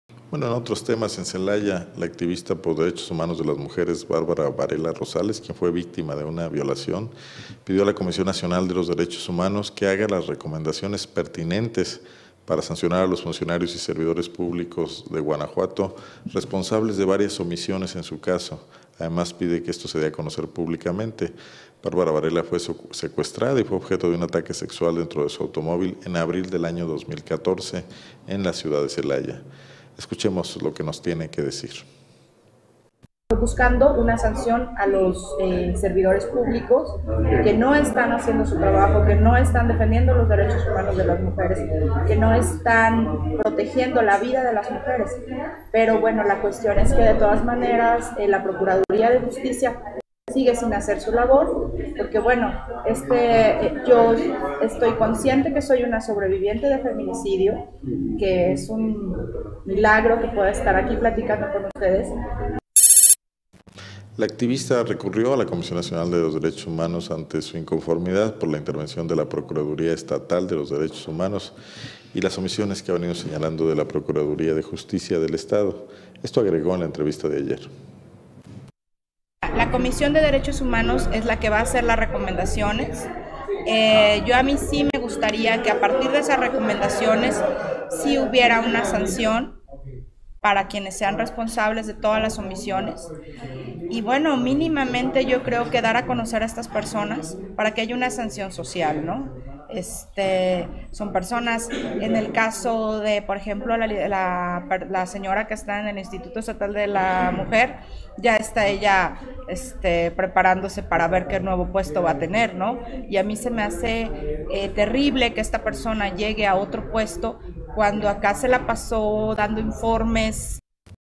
Pide Bárbara Varela se castigue a funcionarios que fueron omisos tras ser agredida
Bárbara Varela Rosales, activista por los derechos humanos, pidió a la Comisión Nacional de los Derechos Humanos (CNDH), que haga las recomendaciones pertinentes para sancionar a los funcionarios y servidores públicos responsables de las omisiones en su caso y que éstas se den a conocer públicamente.
Bárbara Varela fue secuestrada y violada dentro de su automóvil en abril del año 2014 en la ciudad de Celaya.